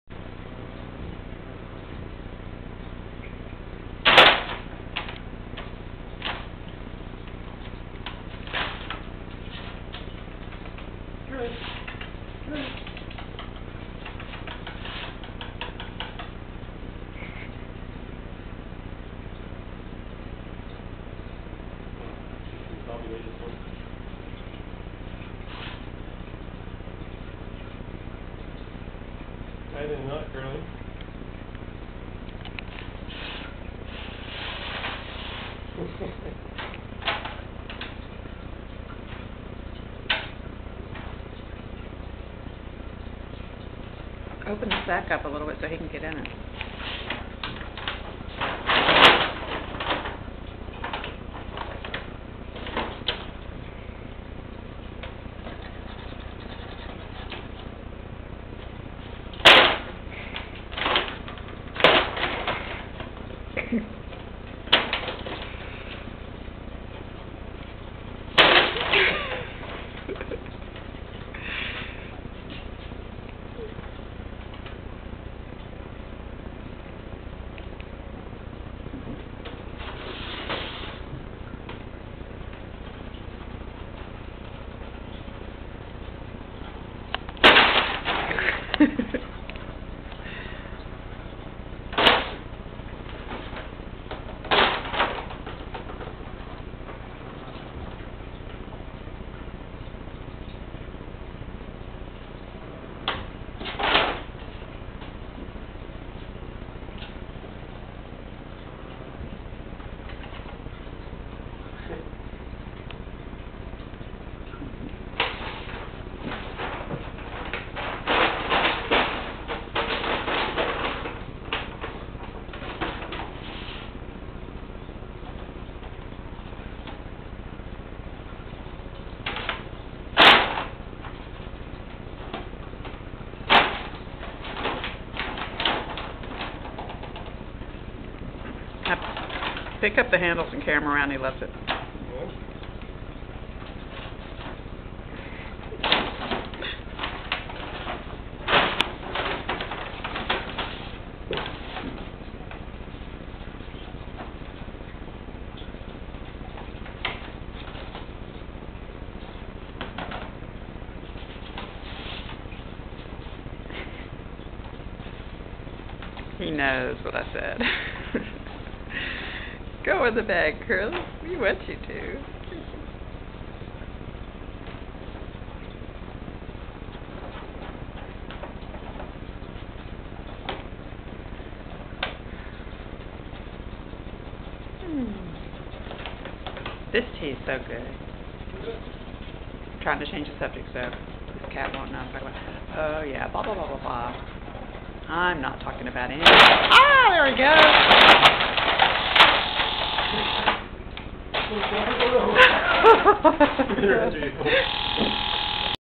Curly the Sphynx Cat plays in a paper bag
I'm sure those of you who miss CURLY will love to see he is doing quite well.